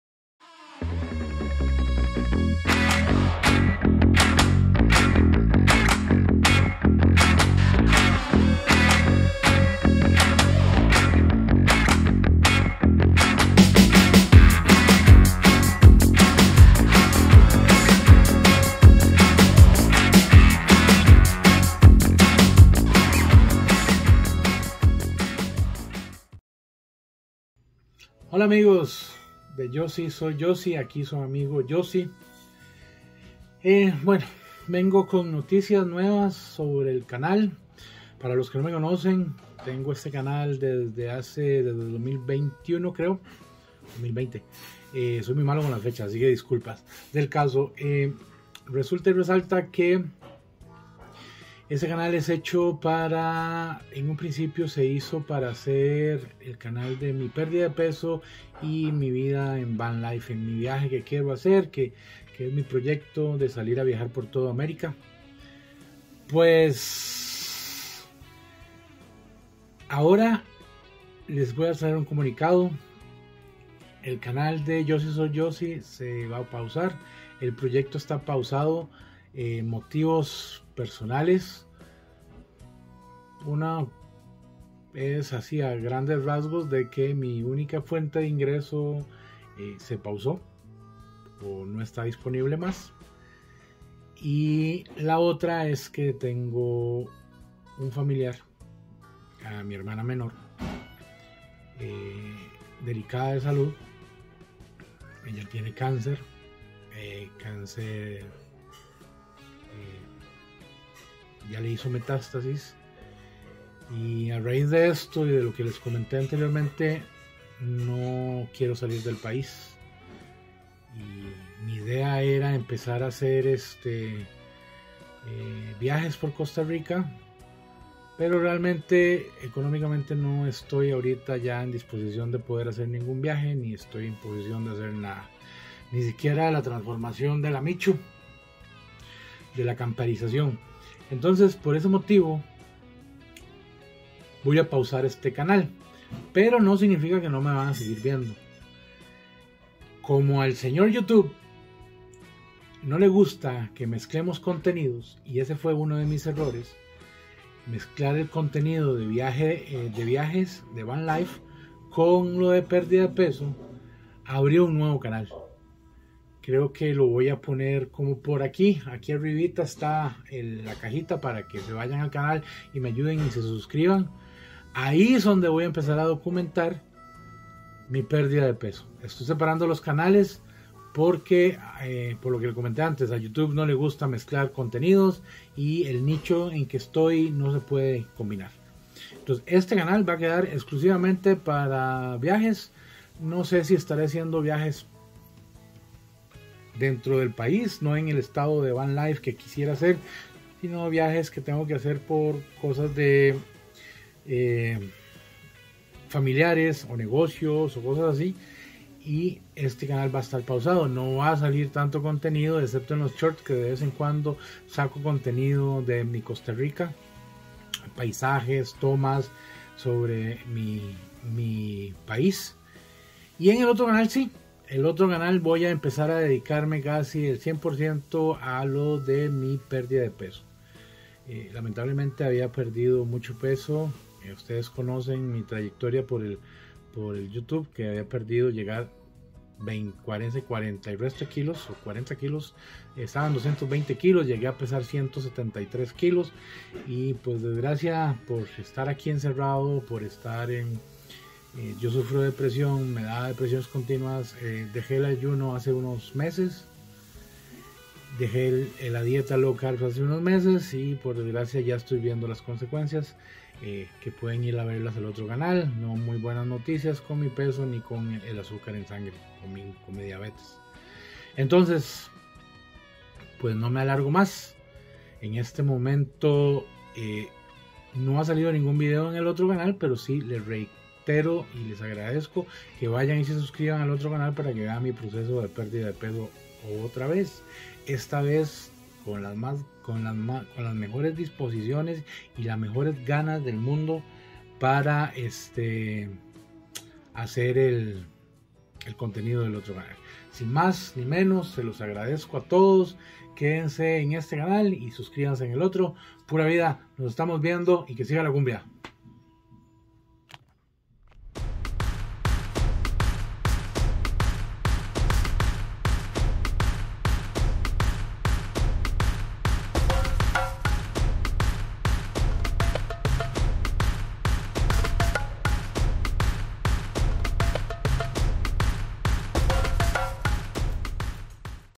Duras Circunstancias: La Pausa de Canal en Yo Si Soy Yossi por Falta de Recursos y Pariente Enfermo
En este video, quiero compartir con todos ustedes la razón detrás de la pausa en mi canal, Yo Si Soy Yossi. Debido a la falta de recursos y la enfermedad de un ser querido, me vi obligado a tomar un descanso temporal. En este anuncio de pausa, quiero abrirme y compartir con ustedes las difíciles circunstancias que estoy enfrentando.

A través de esta historia personal, espero crear conciencia sobre los desafíos a los que muchos nos enfrentamos en la vida. Quiero transmitir un mensaje de perseverancia y lucha, demostrando que, a pesar de los obstáculos, siempre hay esperanza.

Agradezco infinitamente el apoyo y comprensión de mi comunidad en este momento difícil. Vuestras palabras de aliento significan mucho para mí y me dan fuerzas para enfrentar los desafíos que se presentan.

Les pido que compartan este video para que más personas puedan conocer mi situación y, si están en la capacidad, puedan brindar su apoyo de alguna manera. Juntos, podemos hacer una diferencia y superar estas dificultades.

¡Gracias por estar siempre a mi lado! Nos vemos pronto en Yo Si Soy Yossi.

Si alguien tiene ideas o artilugios para la MICHU bienvenidos sean

Si alguien quiere donar algo se les agradecería.